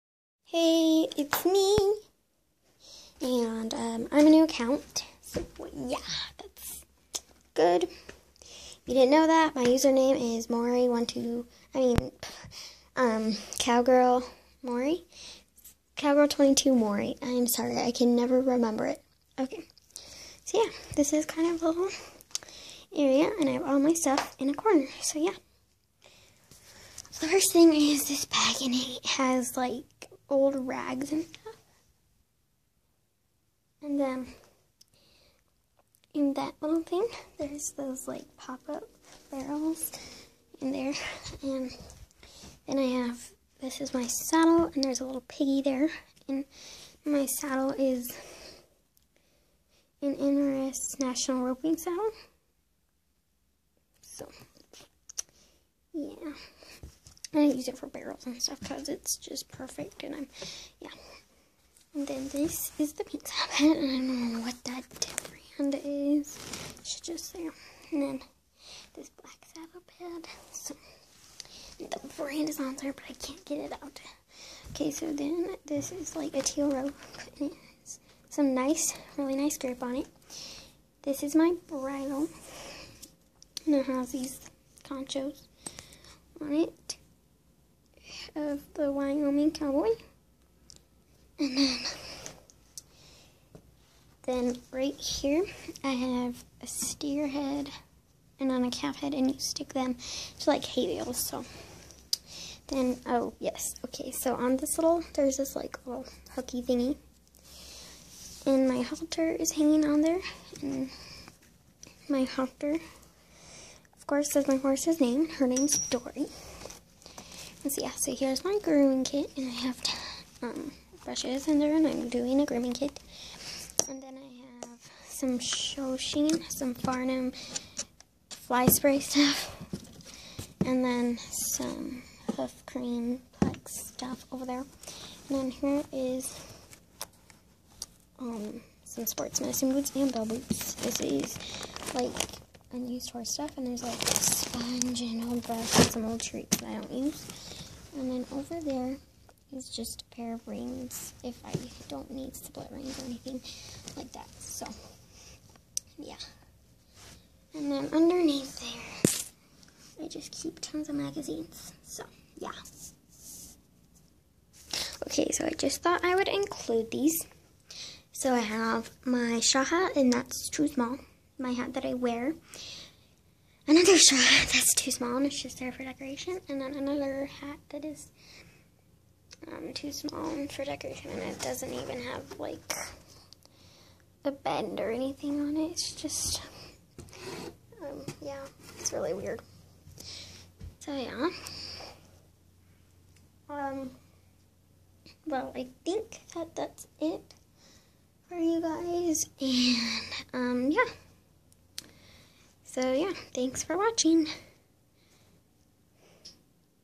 Tack room tour 🐴 💚 👌
Hi there. I am a new channel and I have many videos to share with you. I have two mares. There names are Dori (sorrel mare) and Dollie (really dark brown almost black mare) I am excited to share my videos! I suggest you turn up your volume I am talking very quietly. (I don't know why) 😁😂👌🐴🐴🐎